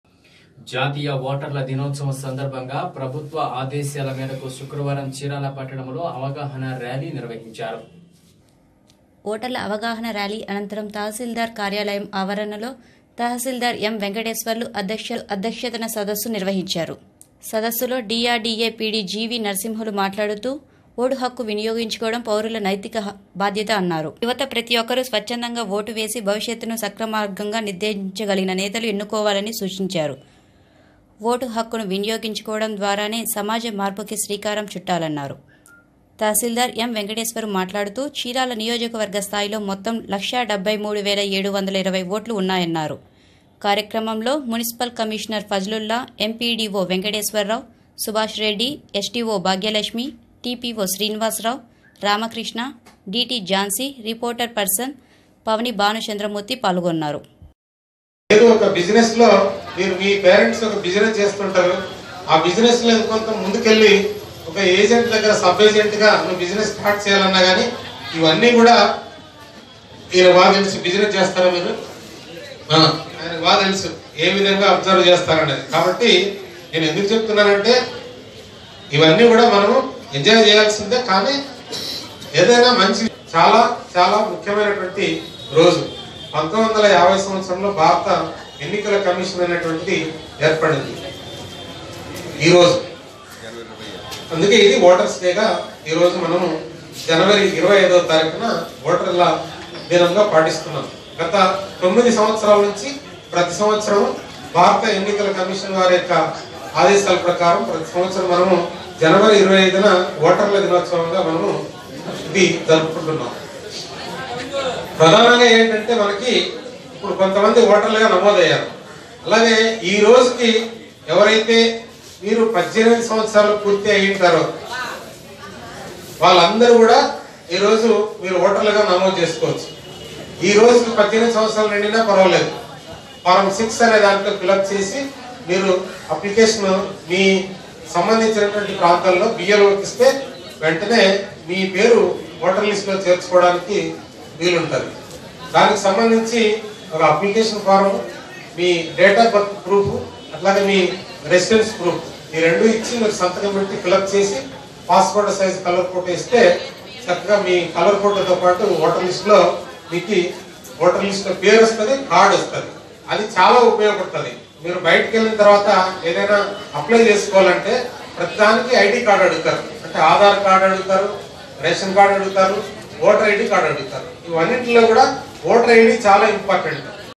CHIRALA :- voters day rally |DN5
జాతీయ ఓటర్ల దినోత్సవం సందర్భంగా ప్రభుత్వ ఆదేశాల మేరకు శుక్రవారం చీరాల పట్టణంలో అవగాహన ర్యాలీ నిర్వహించారు.